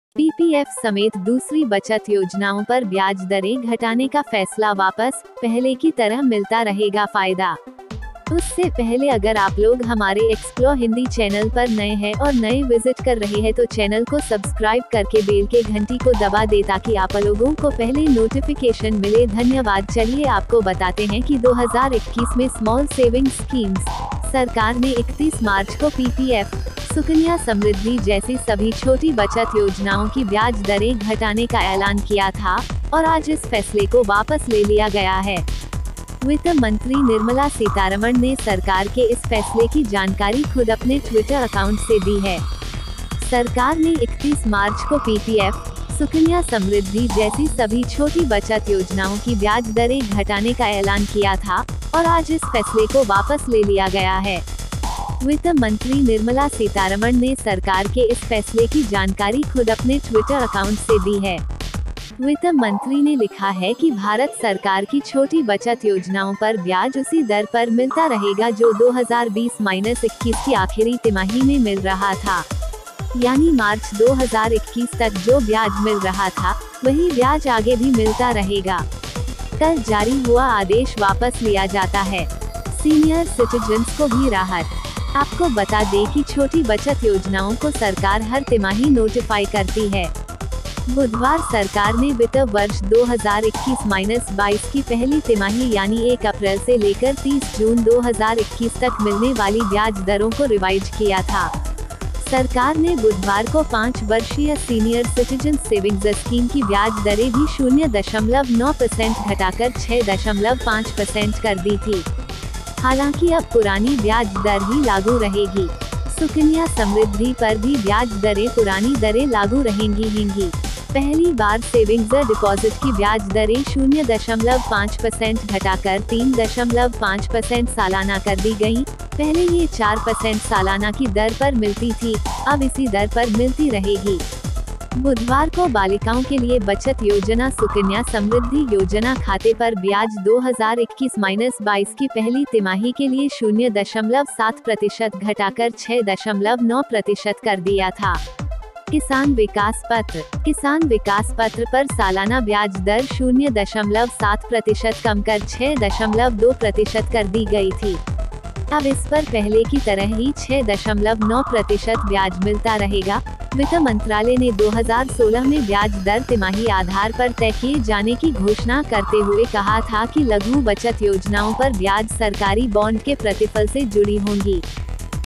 Pm modi ने 31 march2021 को सुकन्या समृद्धिजैसी सभी छोटी बचत योजनाओं की ब्याज दरें घटाने का ऐलान किया
Finance Minister Nirmala Sitharaman ने 31 मार्च 2021 को PPF, सुकन्या समृद्धि जैसी सभी छोटी बचत योजनाओं की ब्याज दरें घटाने का ऐलान किया
Small savings schemes: सरकार ने 31 मार्च को PPF, सुकन्या समृद्धि जैसी सभी छोटी बचत योजनाओं की ब्याज दरें घटाने का ऐलान किया था, और आज इस फैसले को वापस ले लिया गया है. वित्त मंत्री निर्मला सीतारमण ने सरकार के इस फैसले की जानकारी खुद अपने Twitter अकाउंट से दी है.